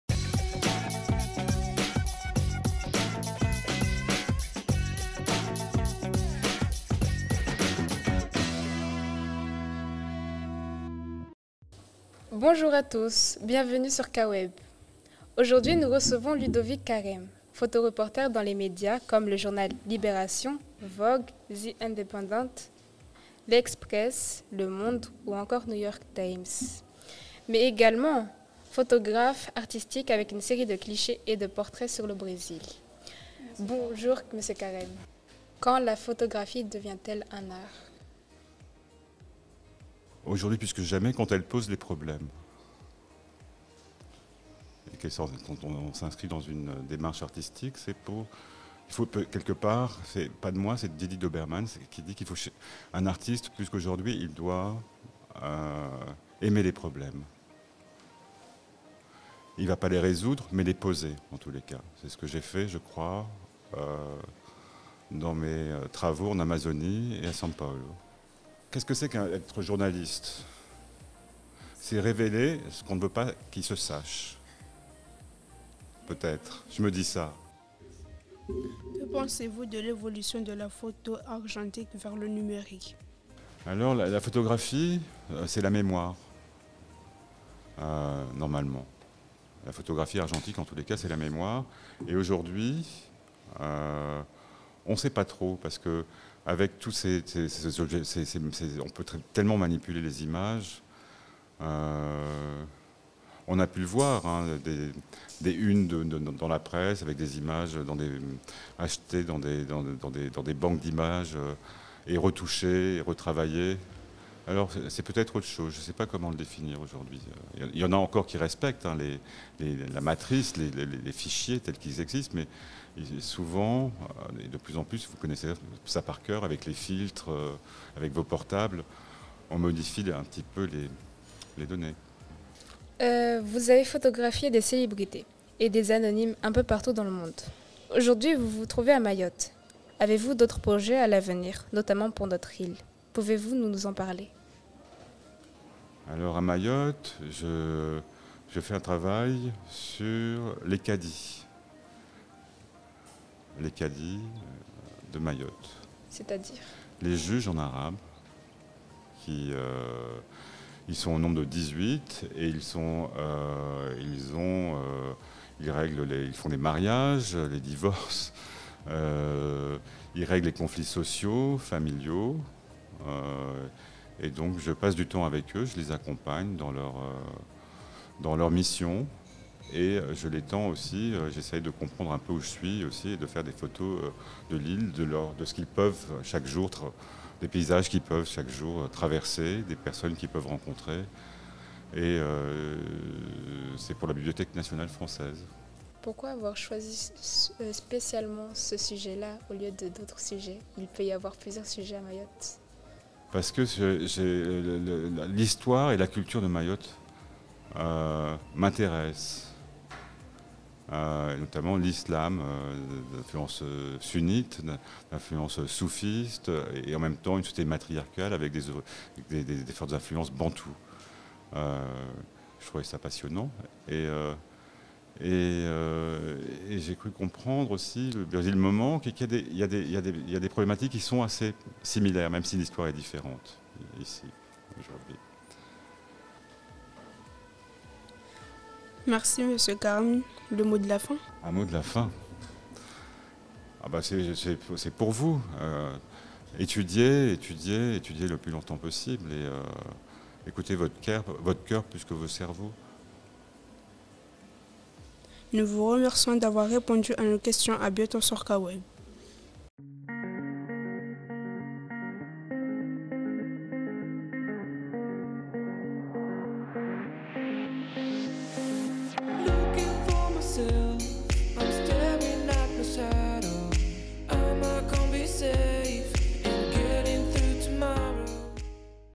Interview de Ludovic Carème - Podcast
Vendredi 23 septembre, les élèves de la 3e journalisme ont reçu Ludovic Carème, photographe ayant travaillé notamment pour des journaux comme « Libération », « Le Monde », « Vogue », « L’express », mais aussi « New York Times » (Etats-Unis), « The independant » (Angleterre) et « La repubblica » (Italie).
Aujourd’hui, il se consacre davantage à la photographie documentaire ; il pratique le portrait, dont celui de David Lynch, Woody Allen, John Malkovich, Jesse Einsenberg, Morgan Freeman ou encore Vincent Cassel, ainsi que nombre d’anonymes, notamment dans les favelas de Sao Paulo, ainsi que la photographie artistique.
Nos journalistes en herbe ont interviewé l’artiste aux micros de Caweb, notre média scolaire, sur son métier, ses expériences, son art, ses œuvres et ses projets futurs. Une fois les micros coupés, une belle rencontre en toute simplicité faite de questionnements et d’échanges s’est déroulée, ouvrant un peu plus les horizons de nos élèves sur le vaste monde qui les entoure : une expérience très enrichissante pour nos collégiens. Nous souhaitons un bon retour à Ludovic Carème et au plaisir de l’entendre à nouveau sur notre Webradio.